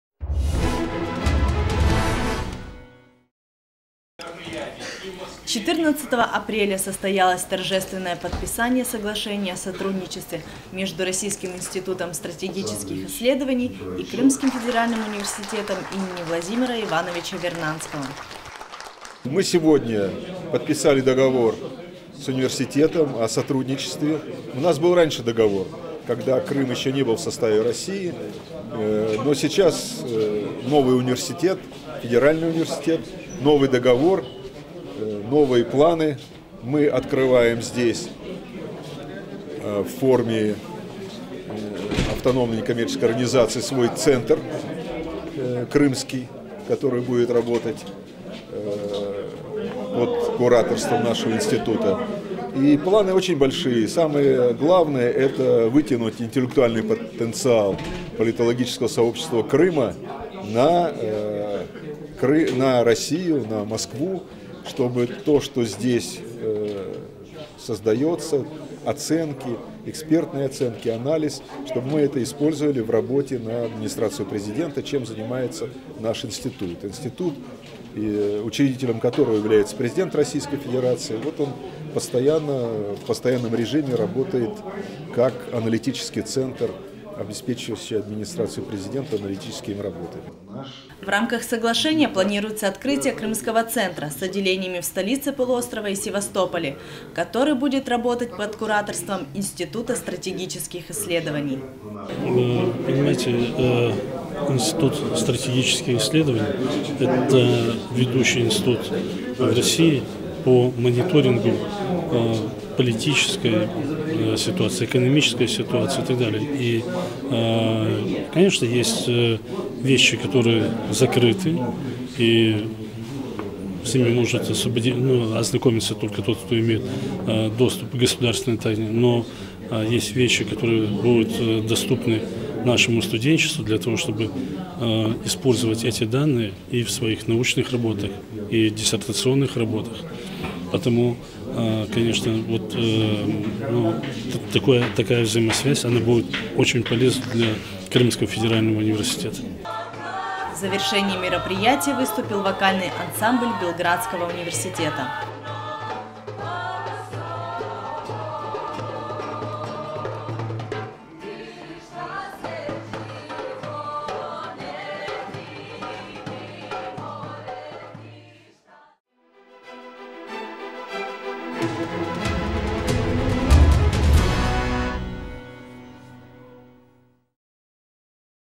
Подписание соглашения о сотрудничестве между РИСИ и КФУ им. В.И. Вернадского 14.04.15
Телевидение Крымского федерального университета им. В.И. Вернадского

14 апреля в Крымском федеральном университете им. В.И. Вернадского состоялось торжественное подписание соглашения о сотрудничестве между "Российским институтом стратегических исследований" и Крымским федеральным.

В рамках соглашения планируется открытие Крымского  центра, который будет работать под кураторством института стратегических исследований.